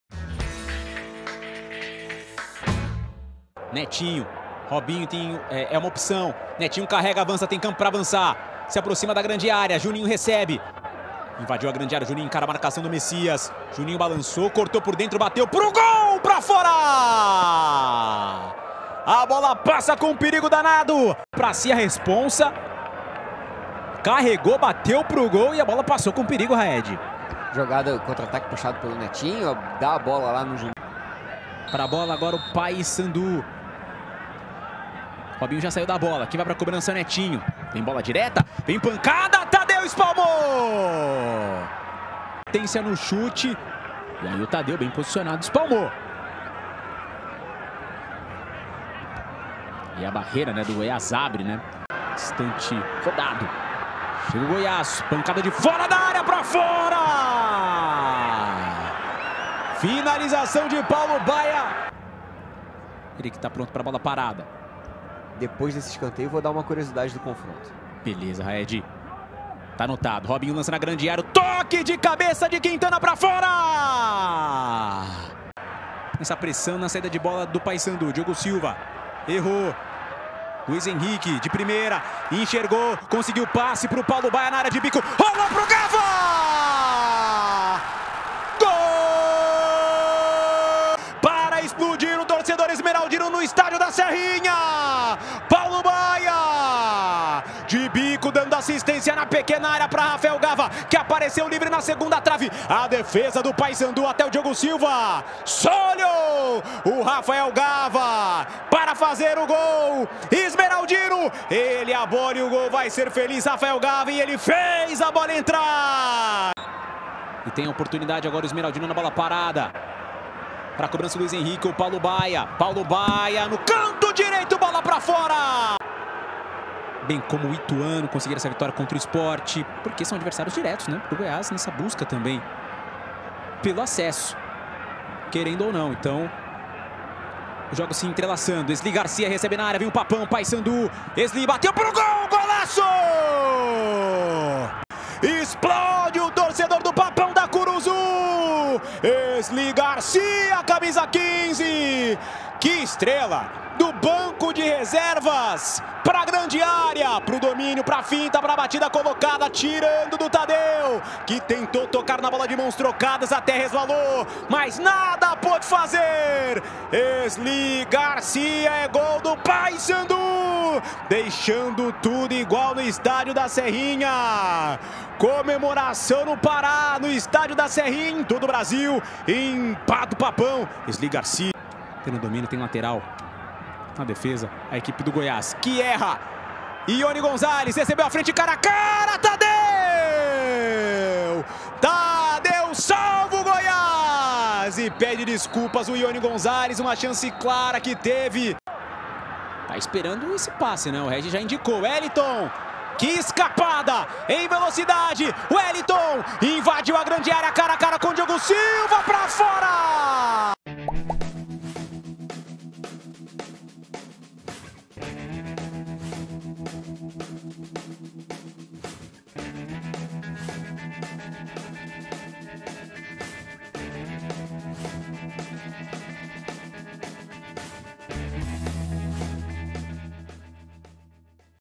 GOIÁS 1 X 1 PAYSANDU | MELHORES MOMENTOS | 24ª RODADA BRASILEIRÃO SÉRIE B 2024 | ge.globo
Em jogo equilibrado, Goiás e Paysandu empatam pela Série B. Equipe paraense conquista ponto importante fora de casa, mas segue sem vencer a oito jogos. Já os Esmelradinos sobe uma posição na tabela